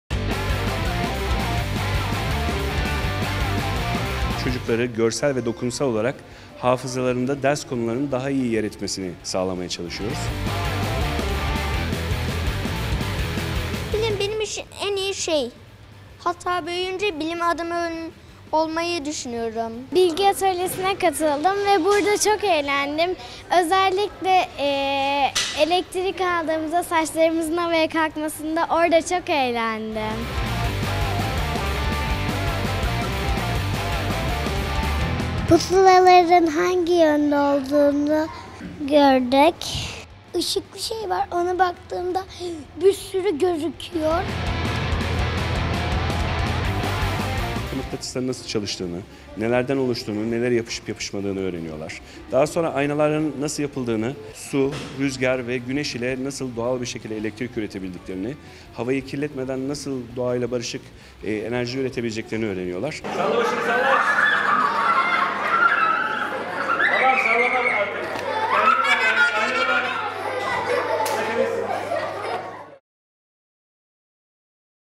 Aklın Yolu Bilim Atölyesi
Çocuklar bilimin eğlenceli yüzüyle Yarıyıl Şenlikleri'nde tanışıyor.

Geleceğin bilim insanları Beylicium Avm'de buluşuyor.

Aklın Yolu Bilim Atölyesi'nde bilimi deneylerle öğreniyorlar.

Bilimin sihirli yolculuğuna çıkan çocuklar bilimsel keşifleri ve icatları yakından tanıyor.

Atölyeye katılmak isteyenler 28 Ocak pazar gününe kadar Beylicium Avm'yi ziyaret edebilirier.